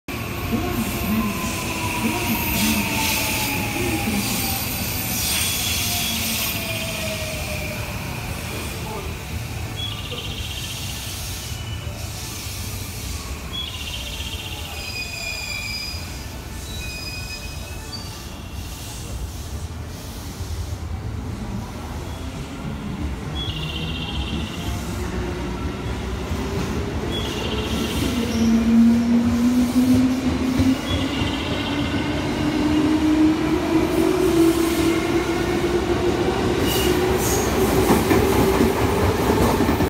【大阪環状線内回り】221系8両 大和路快速加茂行き 森ノ宮発車！
2018年10月24日（水）森ノ宮駅にて撮影

↓大阪環状線のウィキペディア

大阪環状線 - Wikipedia 大阪環状線 - Wikipediaの画像大阪環状線（おおさかかんじょうせん、英名：Osaka Loop Line）は、大阪府大阪市内の大阪駅 - 西九条駅 - 天王寺駅 - 京橋駅 ...